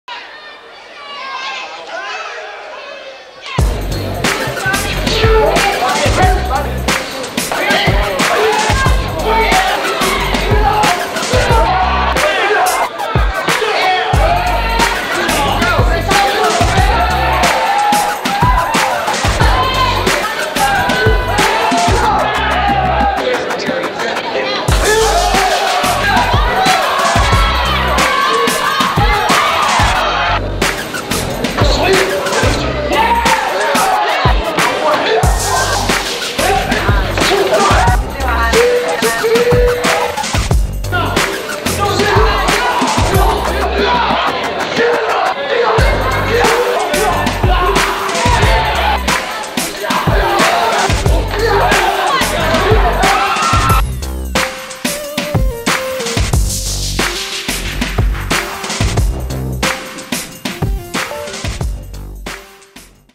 Johnny Linebarger Greatest Hits Old School 1985 USKA Worlds
Johnny Linebarger, director of the KoSho Martial Training Institute, with great hits from the 1985 USKA World Championships in Houston, Texas.